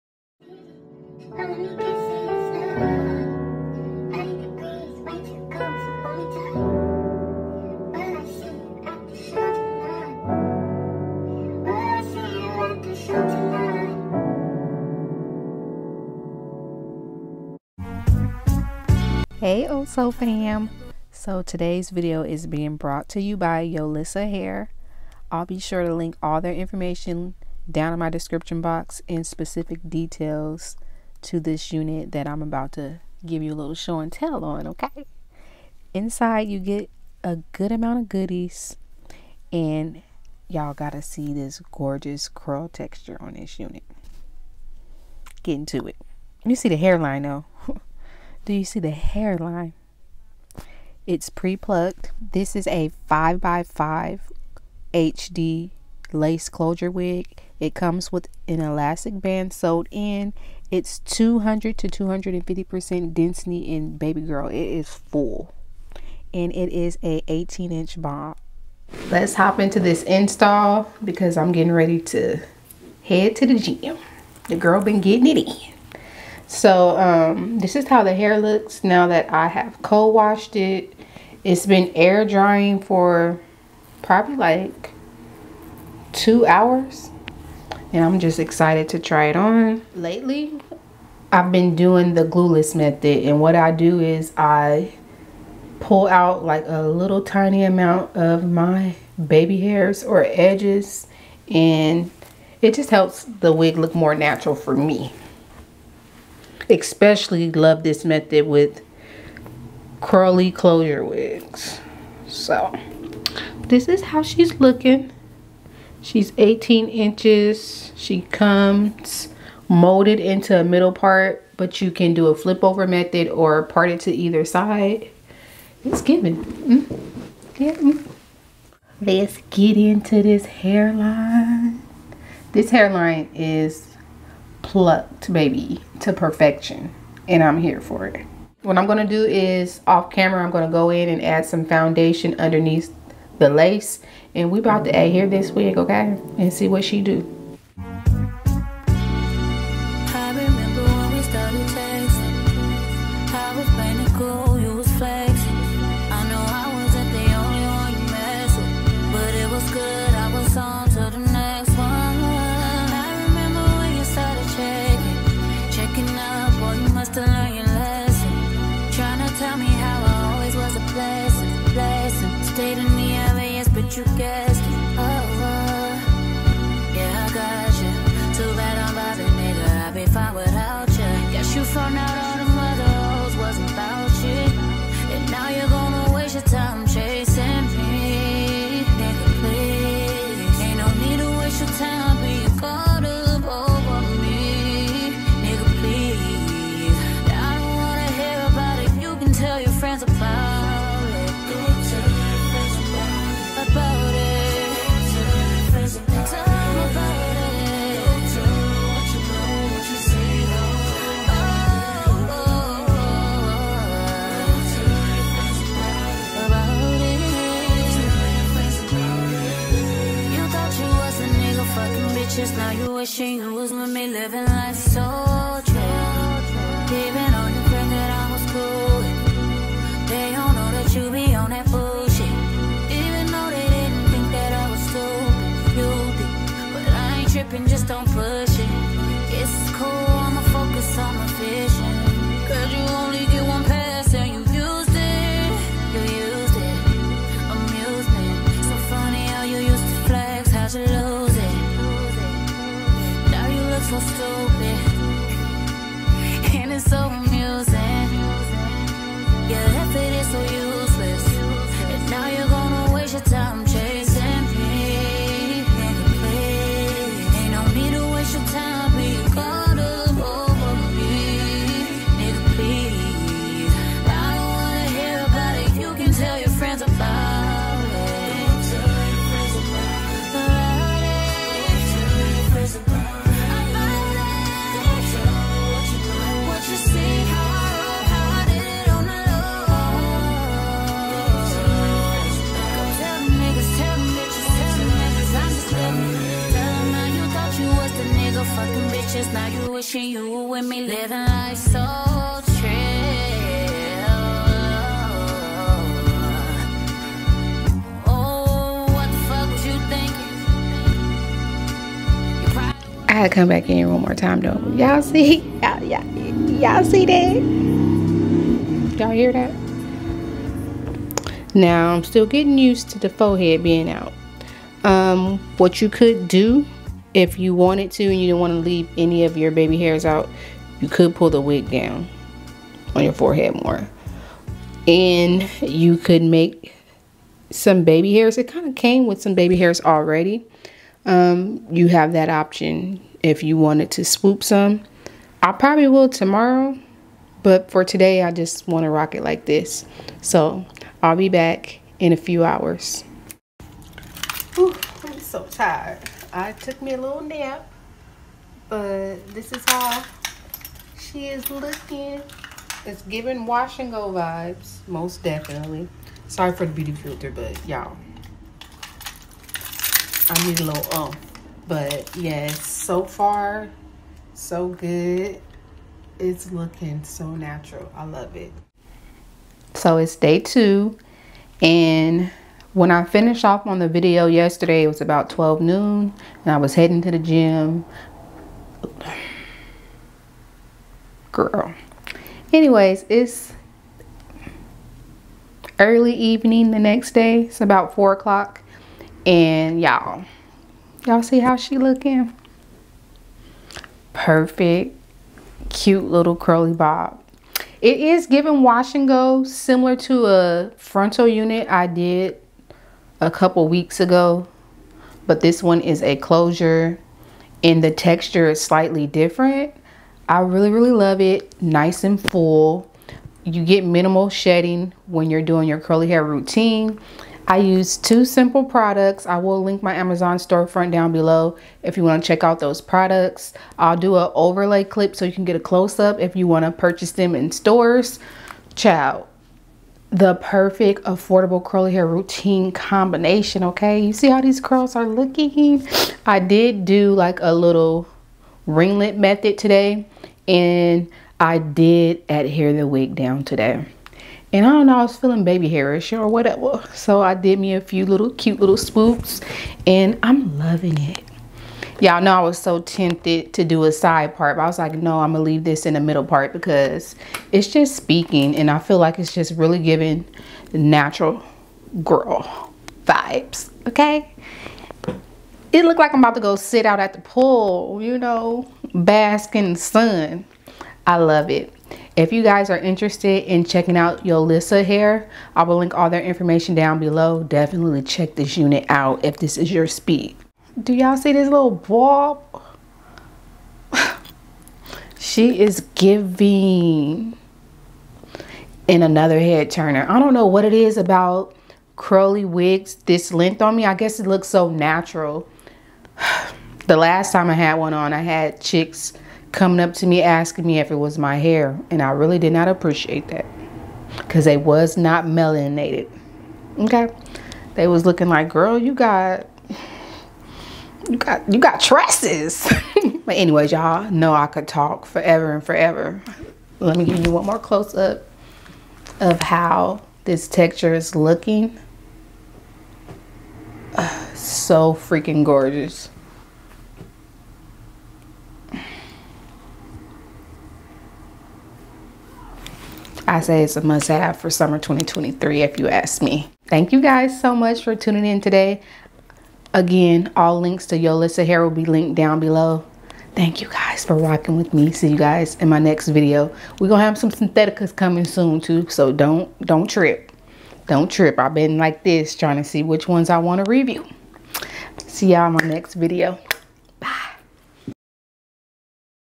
👉Must Have 2023 Bob Season! Pre Plucked Short Curly Bob Wig Install | Yolissa Hair
Hey Cuzinnss!! It's bob season and what better way to celebrate than to do a curly bob show & tell.
This curly closure wig is 100% glueless and perfect for beginners. =)

Amazon Store Front

Hair Info: yolissaShort Curly 5x5 HD Lace Closure Wig 18 inches